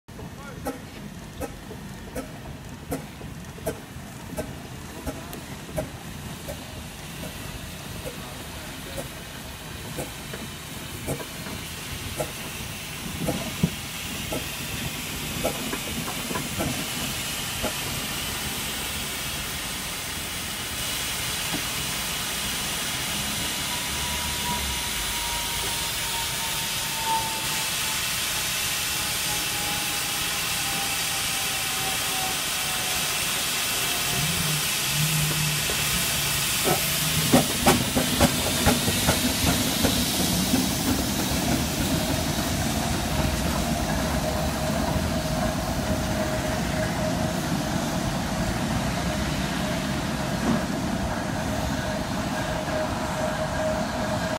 Wallis Traction engine Wildfire in Bordon in 2018
Wallis Traction engine Wildfire in Bordon in 2018 on her way to Kingsley Steam Rally